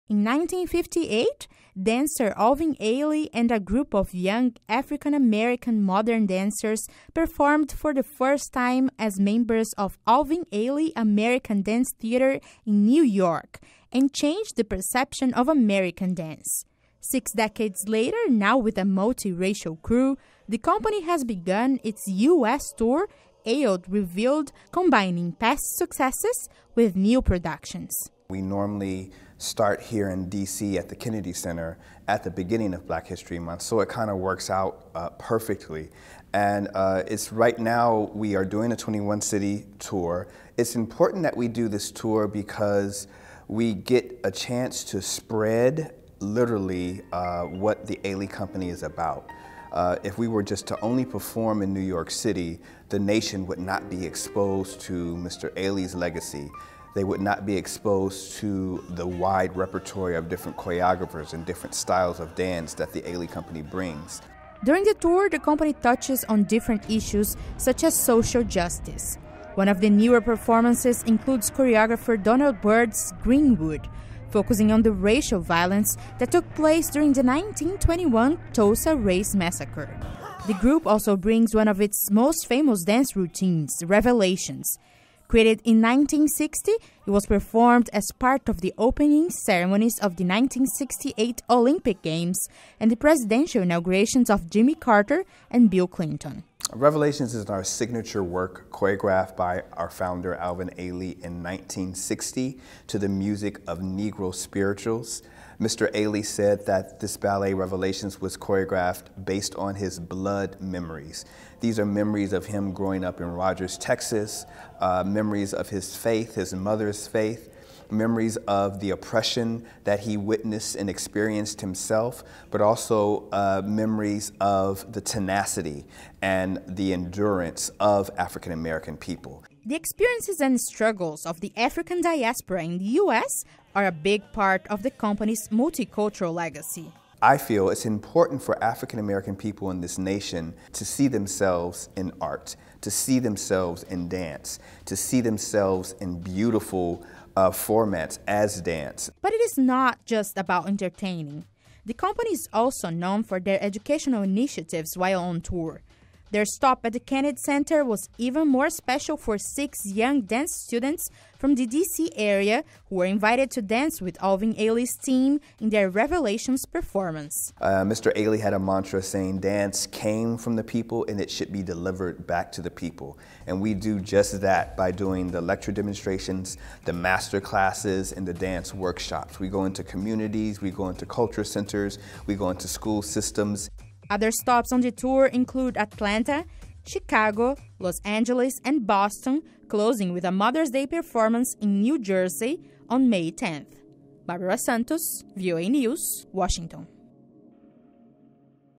Alvin Ailey dance company kicks off U.S. tour "Ailed Revealed” in DC
For more than 60 years, the Alvin Ailey dance company has been changing the look of modern dance with multi-racial performers and community outreach beyond the stage. Red Carpet’s Barbara Santos caught up with the group at the Kennedy Center here in Washington where they are kicking off their U.S. tour "Ailed Revealed.”